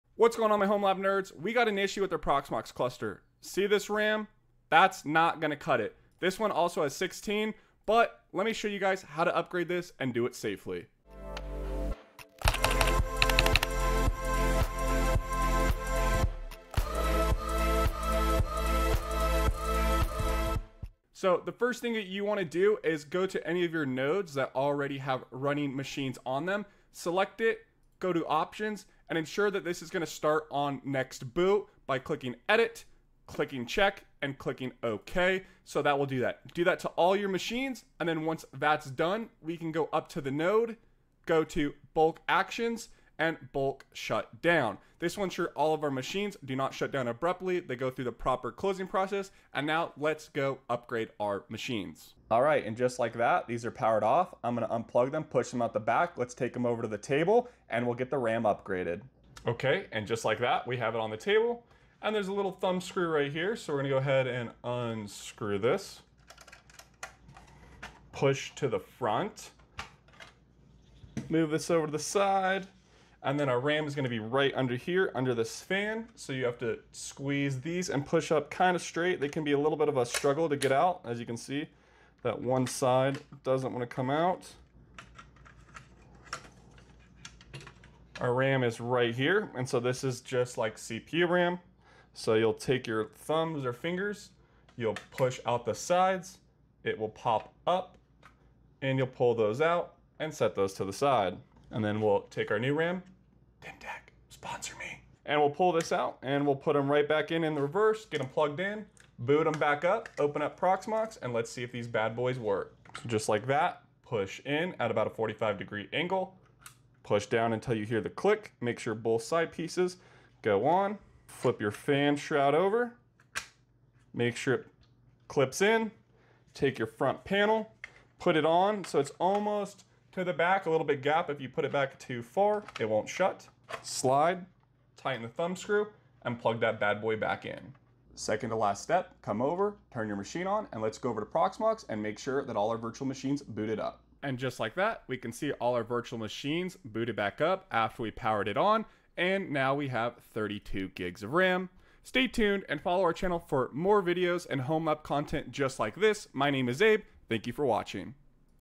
How to Upgrade your Proxmox RAM SAFELY in this 2025 Tutorial Homelab Projects
Hey guys, Abe here; please see the links below to help you throughout this course.

Timestamps: